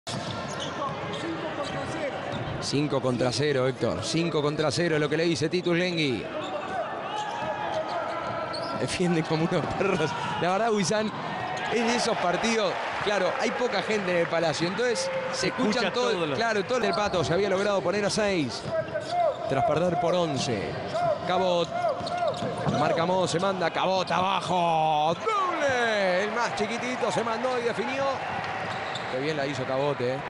LUB: Marcos Cabot ataca el aro. Defensor Sp 76-71 Bigua 28/02/2014
"DEFENSOR SPORTING 76 BIGUÁ 71
En Palacio Peñarol 28/2
Árbitros: Adrián Vázquez, Roberto Mouradian, Diego Borghini
Por cuartos: DSC 21-13, DSC 32-24, DSC 61-51
   Defensor Sporting contuvo una reacción de último momento de Biguá, que llegó a ponerse a dos puntos a falta de 1.50, pero triple de Sebastián Izaguirre consolidó definitivamente el triunfo que pone a su equipo con ventaja de 1-0 en el inicio de los cuartos de final de la Liga Uruguaya.
   Un encuentro parejo durante los seis primeros minutos hasta que Defensor Sporting con un 8-0 sacó una ventaja de 8 puntos (19-11) en el cuarto inicial, en base a una fuerte presión defensiva, y extendió su ventaja al iniciarse el siguiente período a 11 puntos (24-13).
   A partir de allí el resto del partido pareció un calco de Biguá poniéndose a cinco o seis puntos y DSC distanciandose cada vez, llegando a sacar un máximo de 13 puntos (49-36) a falta de 4.42 en el tercer cuarto.
   En el último período se mantuvo el mismo molde de cinco o 6 puntos de ventaja y la recuperación de DSC, hasta que a falta de 1.50 Biguá se puso a un doble (68-66) y el triple de Izaguirre comenzó a definir el encuentro.
   Los mejores goleadores de DSC fueron con 15 puntos Ruben Garces y Federico Alvarez con 15, mientras Sebastián Izaguirre anotó 12 puntos con &7$ en triples 6 rebotes y 3 asistencias. En Biguá Chris Moss marcó 25.
   Volveran a enfrentarse el miércoles 12 de marzo (21.30) en la serie al mejor de cinco.
   (Comentarios de Ernesto Reistz)" Urubasket.com